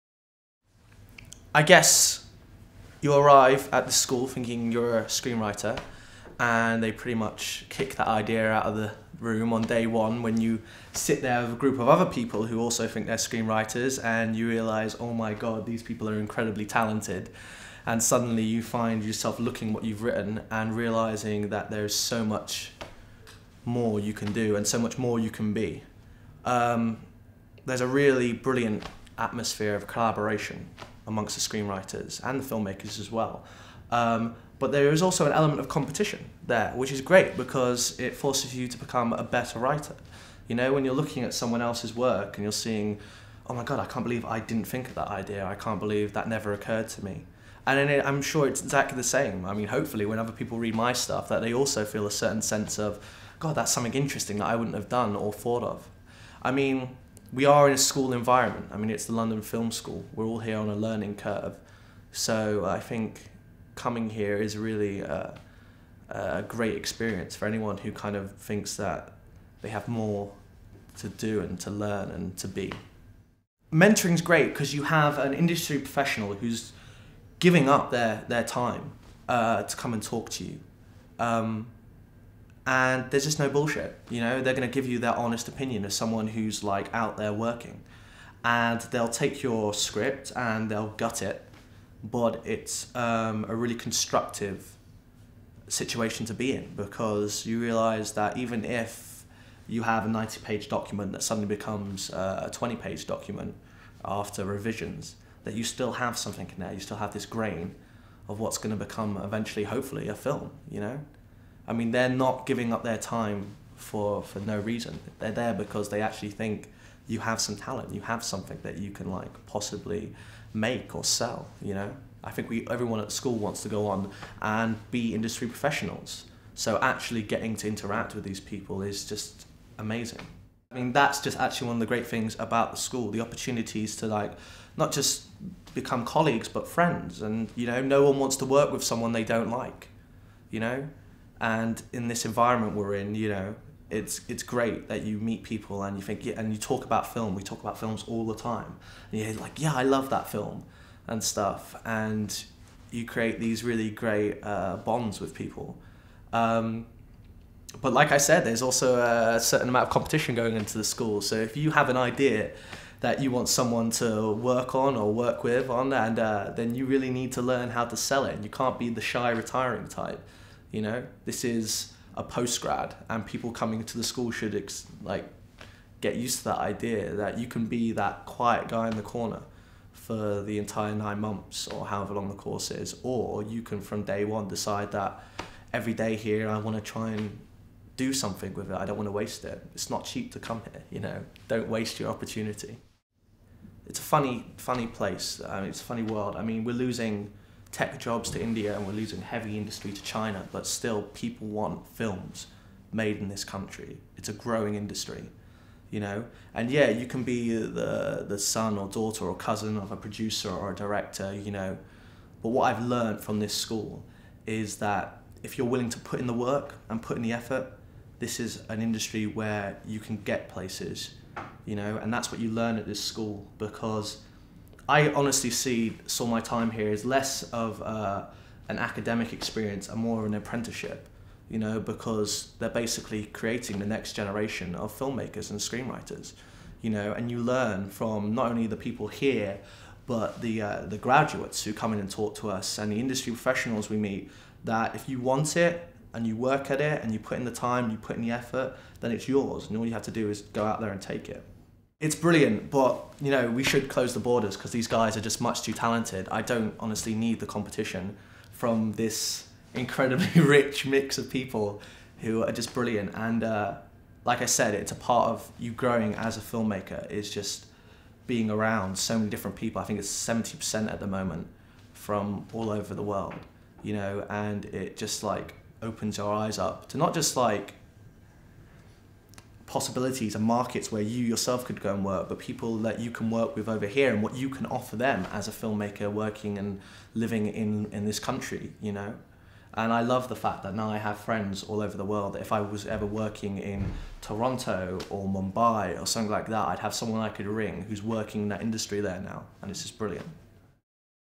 LFS GRADUATE TESTIMONIAL - KARIM ISKANDER FLINT
Karim Iskander Flint (UK) 2010 graduate of the MA Screenwriting course speaking about his experience at the LFS during the 1 year MA course.